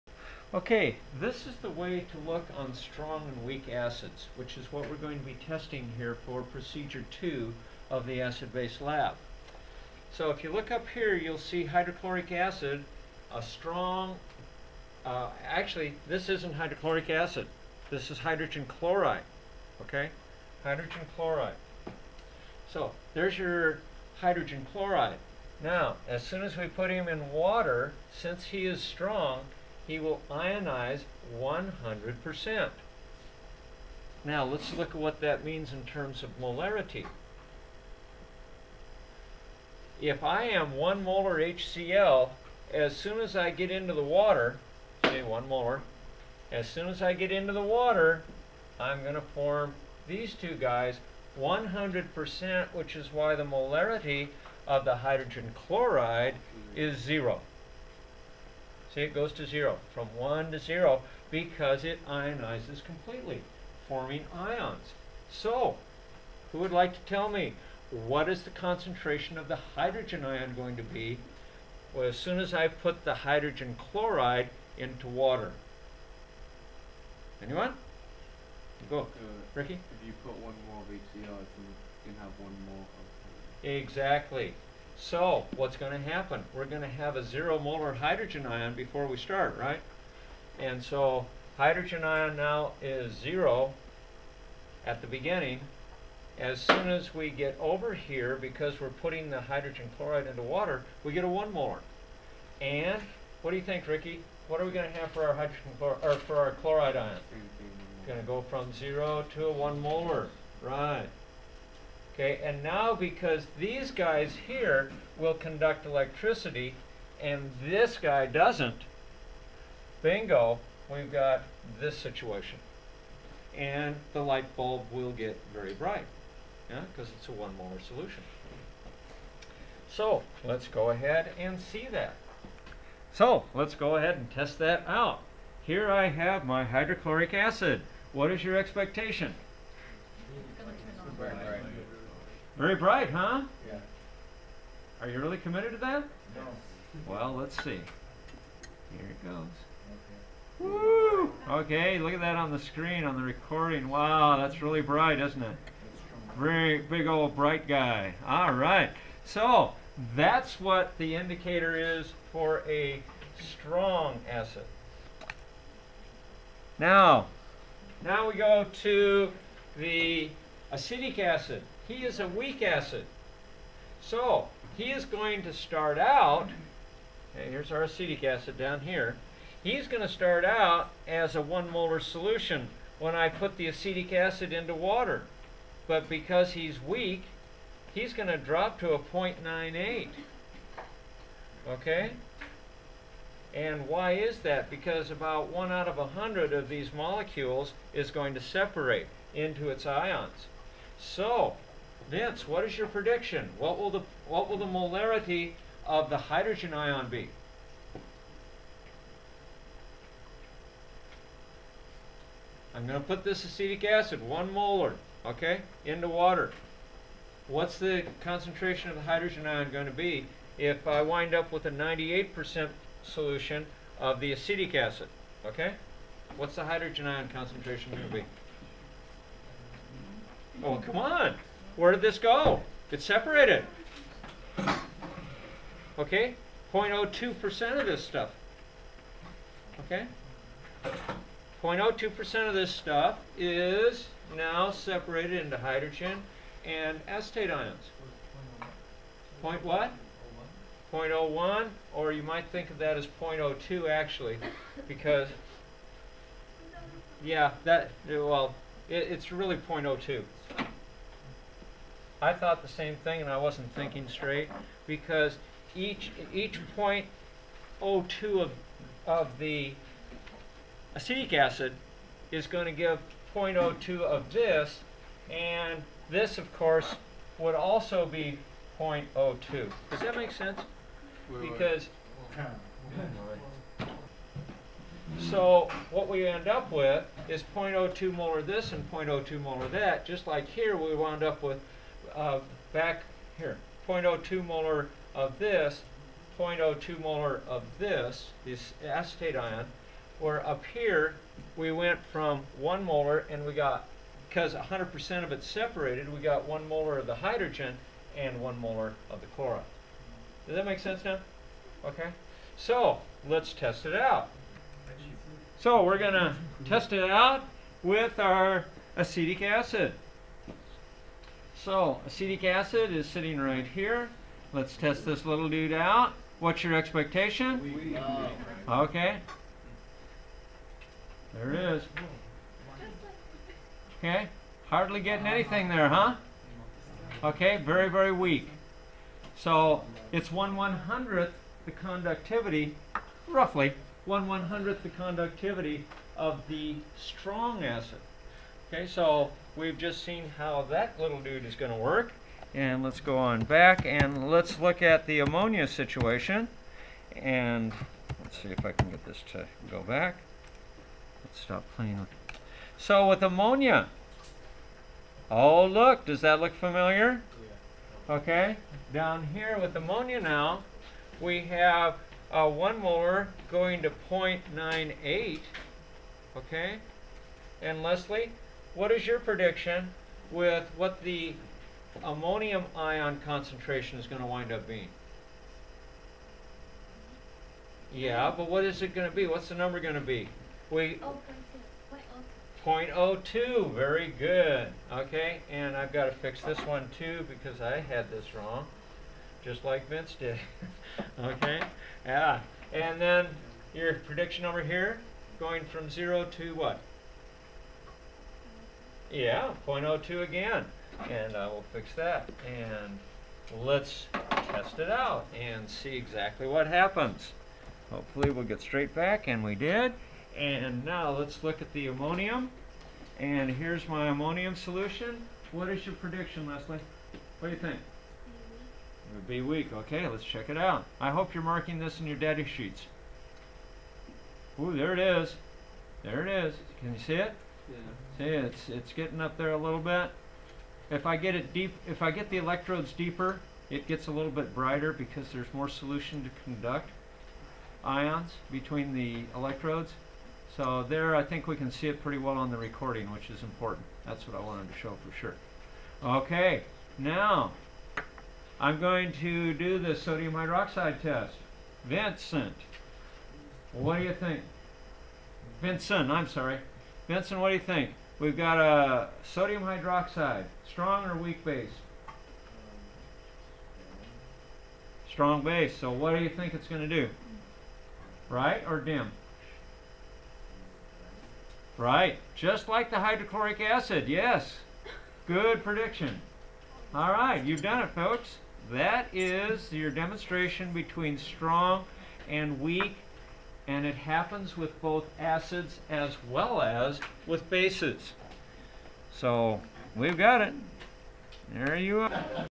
Acid Base Lab - strong vs weak demonstration with Explanation
During this demonstration of Procedure 2 from the Acid Base Lab, I gave an explanation of what Strong vs Weak means when we are speaking of acid or bases.  Strong ionizes 100% (acid or base) and weak acids & bases will only ionize about 1/100 of 1/1000 as much as will the strong acids & bases.  I used a light conductivity tester to demonstrate strong and weak acids (acetic acid and hydrochloric acid) & bases (ammonia and sodium hydroxide).